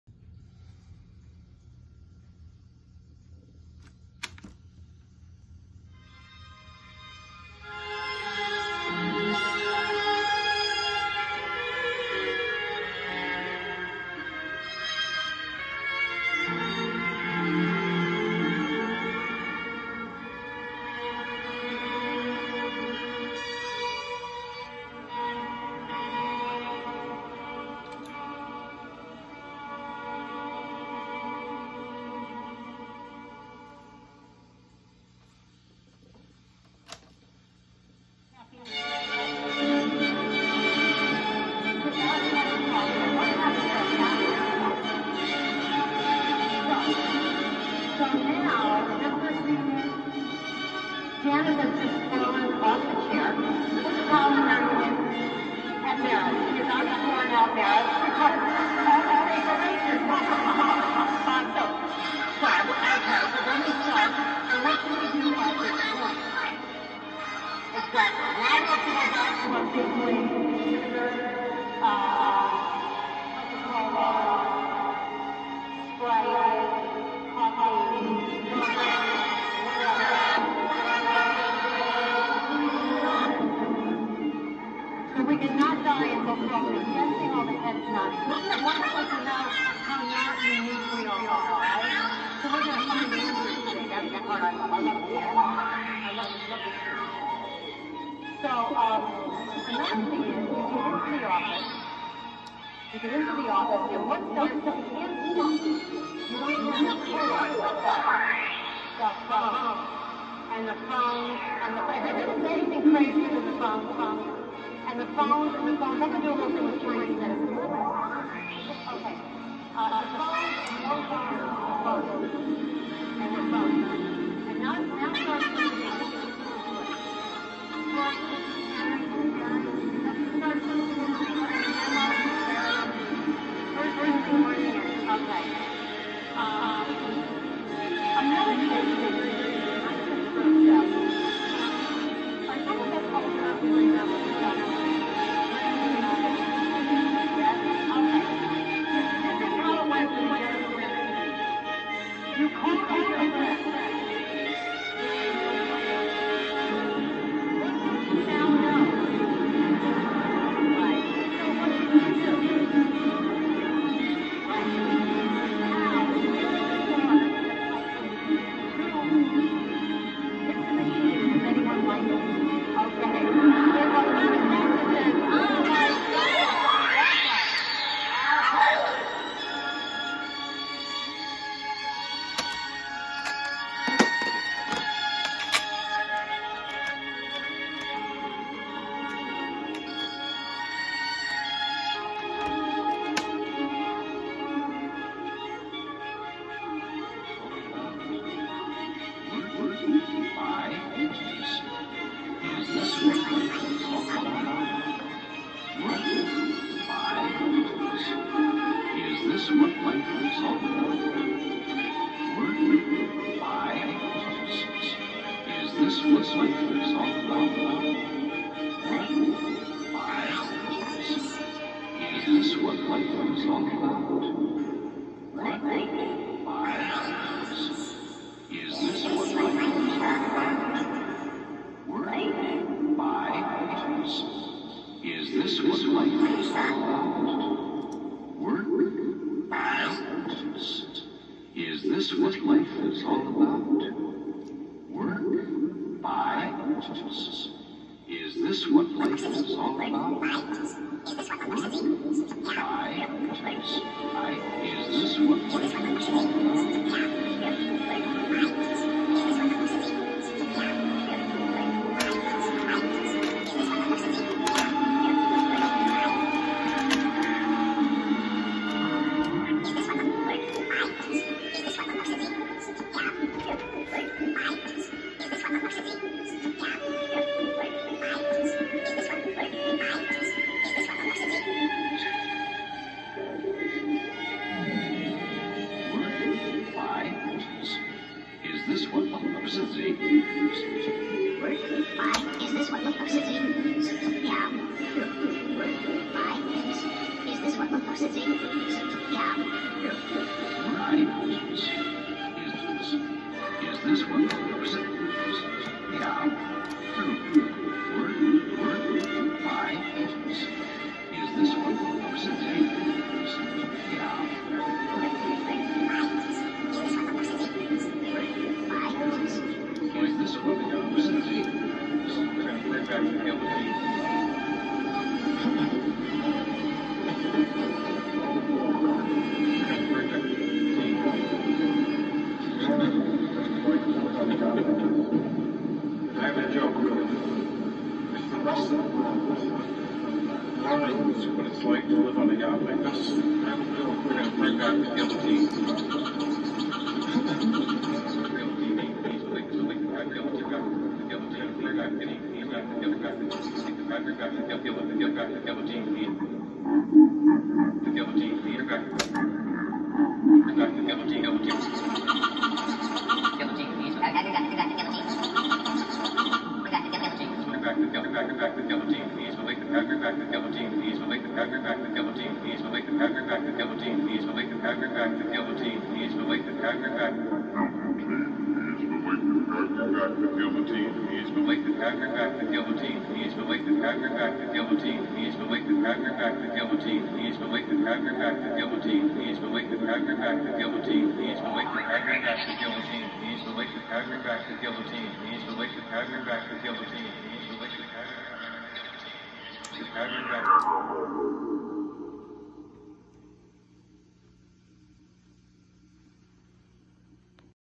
Experimental sound collage using cassette tapes and guitar pedals!
Sound collage using cassette tapes processed by guitar pedals.